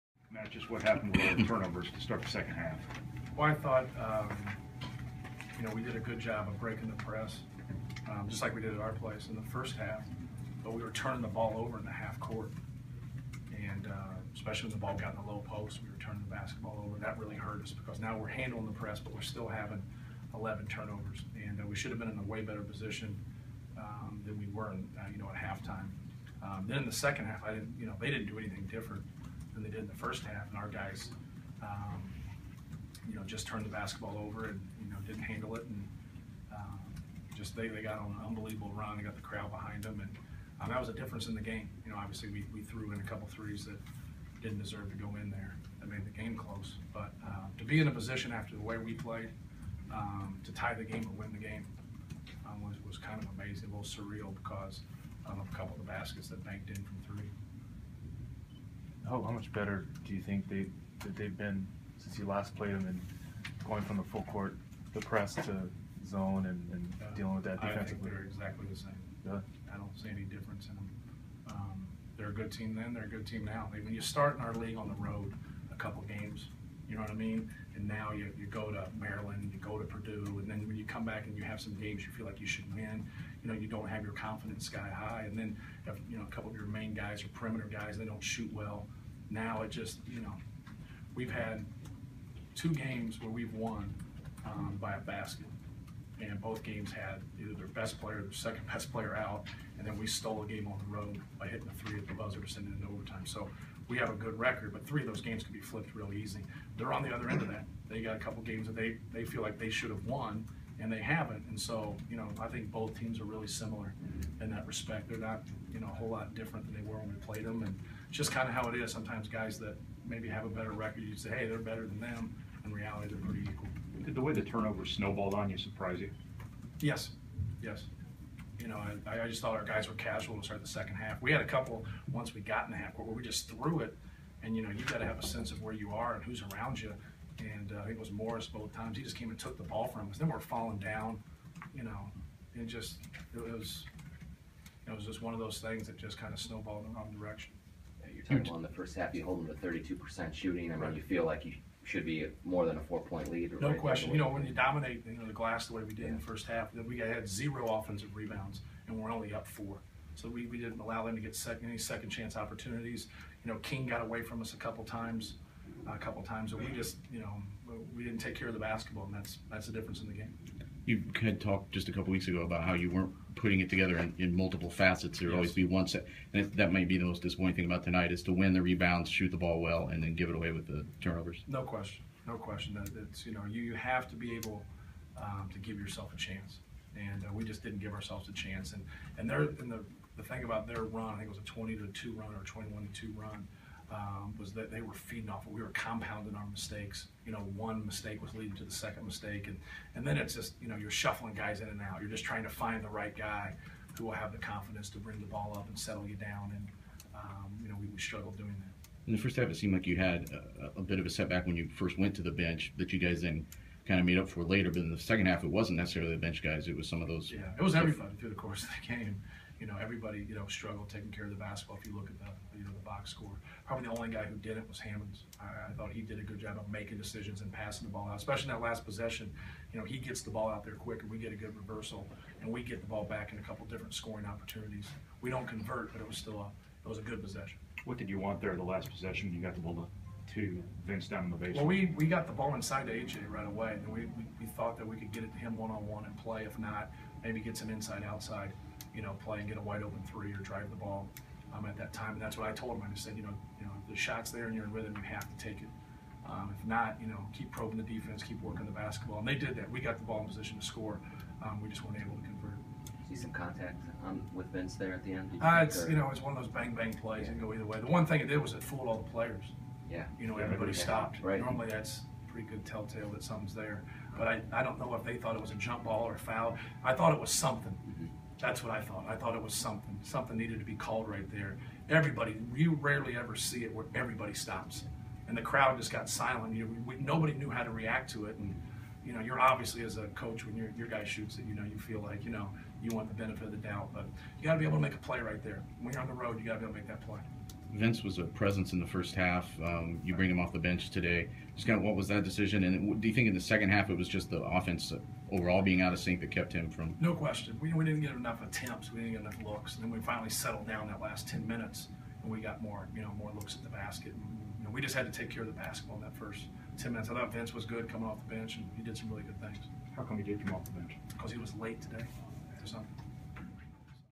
Minnesota 62, Purdue 58: Matt Painter reacts
Purdue coach Matt Painter talks about the Boilers' turnover-plagued loss at Minnesota. (JCOnline.com)